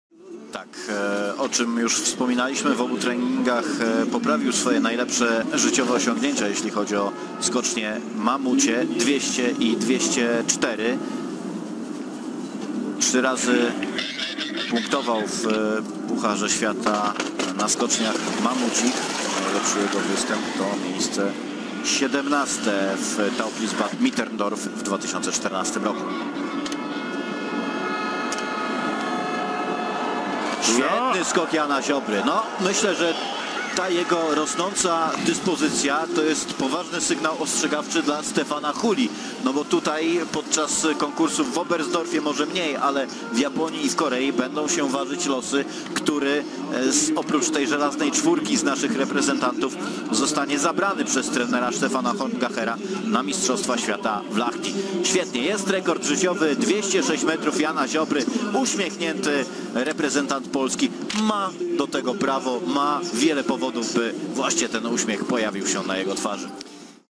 Jan Ziobro 206m NOWY REKORD ŻYCIOWY (Obersdorf 03.02.2017r) Kwalifikacje
Podobało Ci się? Daj łapkę w górę i suba! To bardzo motywuje do dalszej pracy!